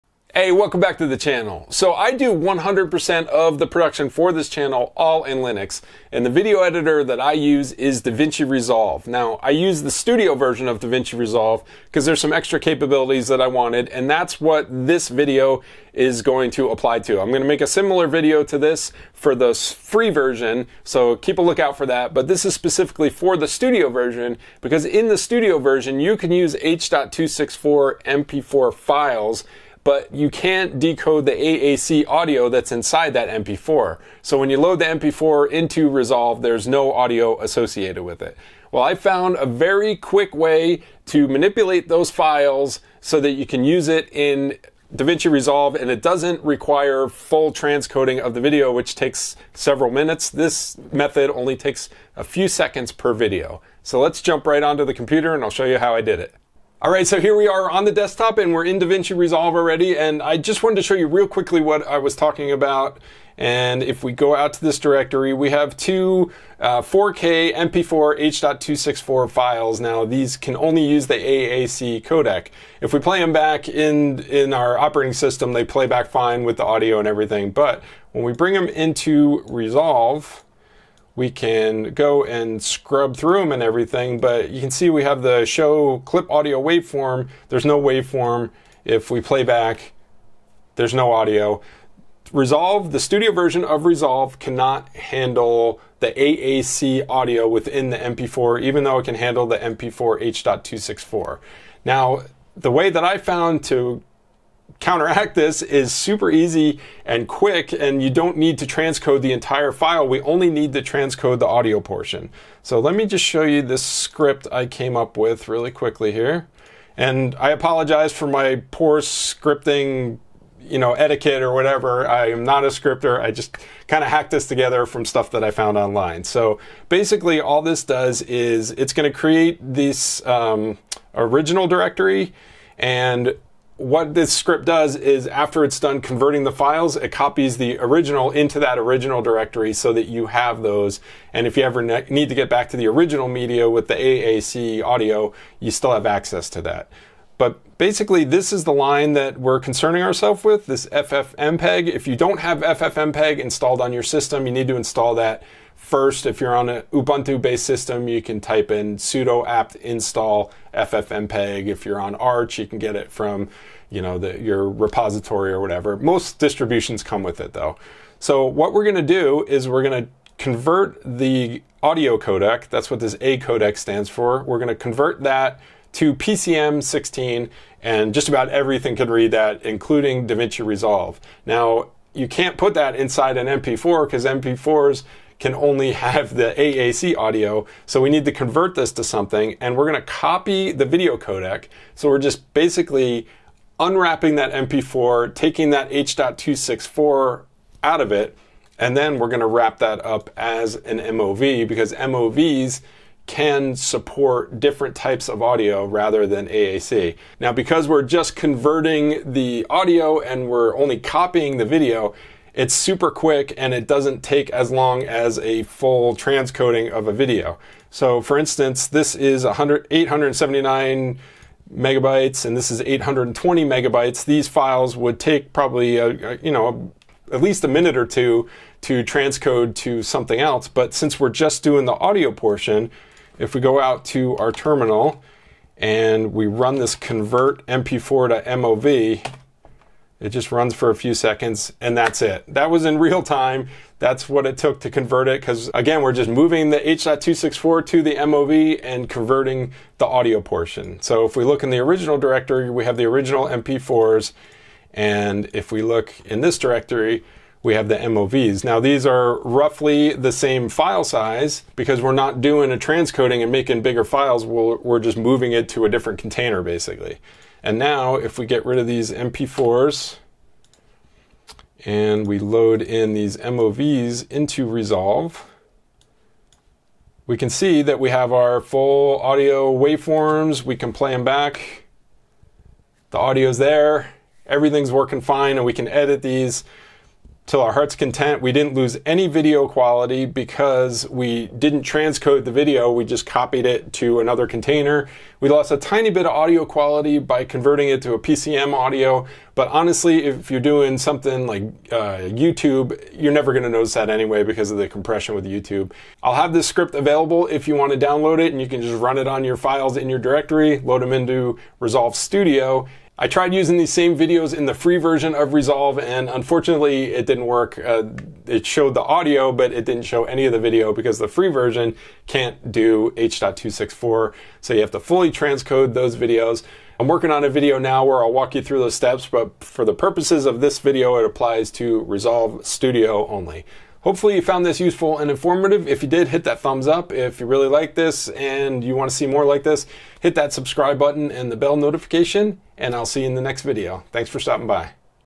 Make MP4 videos work in Resolve Studio on Linux
DaVinci Resolve Studio can handle H.264 MP4 files but cannot play back the audio. In this video I show a quick and easy way to Fix MP4 Audio in DaVinci Resolve Studio on Linux.

* Contents of script shown - Just copy this to a file with .sh extension, set executable and run it in the directory with your MP4 files in it.
set -e
origdir="./original"
shopt -s extglob nullglob

if [ ! -d "$origdir" ];
then
    echo "Creating $origdir directory."
    mkdir "$origdir"
fi

for vid in *.MP4; do
 noext="${vid%.MP4}"
 ffmpeg -i "$vid" -acodec pcm_s16le -vcodec copy "${noext// /_}.mov"
    mv "$vid" "$origdir"
done

☀ Gear used for my videos:
Deity V-Mic D3 Pro
Zoom H1n Handy Recorder